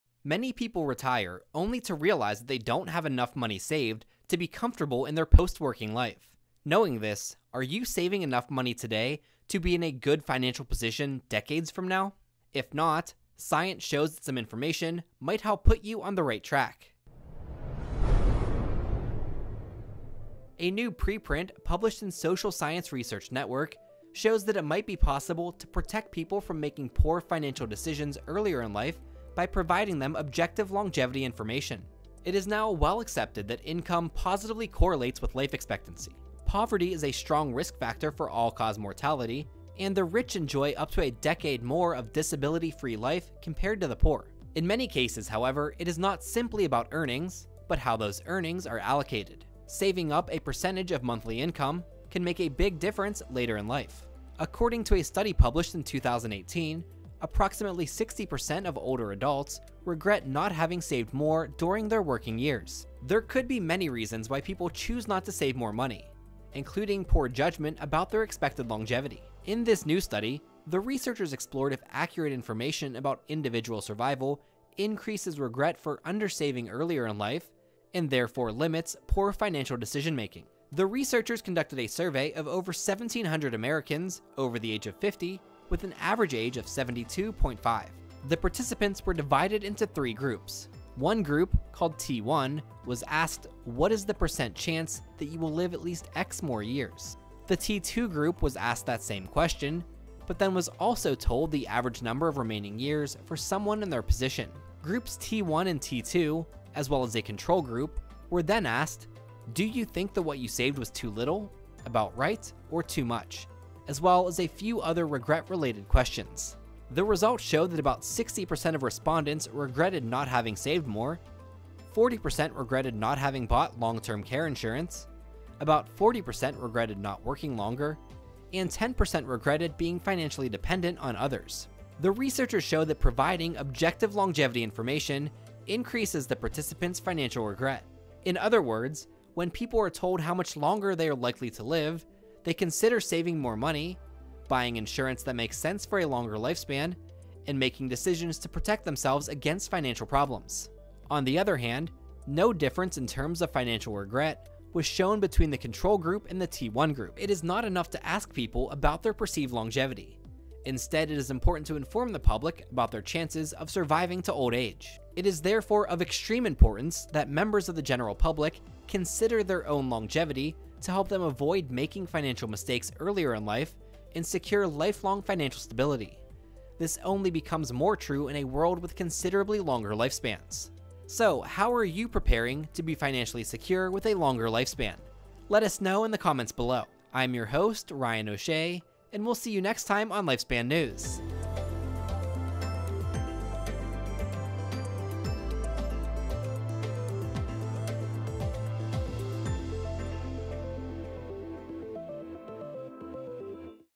Want to Save Money? Understanding Longevity Can Help!
Many people retire only to realize that they haven’t saved enough money to be comfortable for the entirety of their post-working. Knowing this, are you saving enough money today to be in a good financial position decades from now, even post-retirement? If not, some new information might help put you on the right track.

A new preprint published in Social Science Research Network shows that it might be possible to protect people from making poor financial decisions earlier in life by providing them objective longevity information.

It is now well-accepted that income positively correlates with life expectancy. Indeed, poverty is a strong risk factor for all-cause mortality. Moreover, the rich enjoy up to a decade of more disability-free life compared to the poor.

In many cases, however, it is not simply about earnings but in how well these earnings are allocated. Saving up a percentage of monthly income can make a big difference later in life.

According to a study published in 2018, approximately 60% of older adults regret not having saved more during their working years. There could be many reasons why people choose not to save money, including poor judgment about their expected longevity.

In this new study, the researchers explored if accurate information about individual survival probabilities increases regret for undersaving earlier in life and, therefore, potentially prevents poor financial decision making.
The researchers conducted a survey among over 1,700 American adults over the age of 50 with an average age of 72.5.

The participants were divided into three groups. One group, called T1, was asked “What is the percent chance that you will live at least X more years?” The T2 group was asked the likelihood of survival question, but then was also told the average number of remaining years for someone in their position. Groups T1 and T2, as well as a control group, were then asked “Do you think that what you saved was too little, about right, or too much?” and a few other regret-related questions.

The results showed that about 60% of respondents regretted not having saved more, 40% regretted not having bought long-term care insurance, about 40% regretted not working longer, and 10% regretted being financially dependent on others.

The researchers show that providing objective longevity information increases the participants’ financial regret. In other words, when people are given probabilities about how much longer they are likely to live, they consider saving more money, buying insurance that makes sense for such a long lifespan, and making decisions to protect themselves against financial problems throughout their lives.

On the other hand, no difference in terms of financial regret was shown between the control group and the T1 group. Therefore, it is not enough to ask people about their perceived longevity. Instead, it is important to inform the public what their chances are to survive to old age.

This study demonstrates that inaccurate perception of individual longevity prospects seems to be a major reason why older people end up with financial regrets. It is therefore of extreme importance that members of the general public consider their own longevity to help them avoid making financial mistakes earlier in life and secure lifelong financial stability. This only becomes more true in a world with considerably longer lifespans.

PRODUCTION CREDITS
Producer & Editor - Ryan O’Shea
Writer - Larisa Sheloukhova, Ryan O’Shea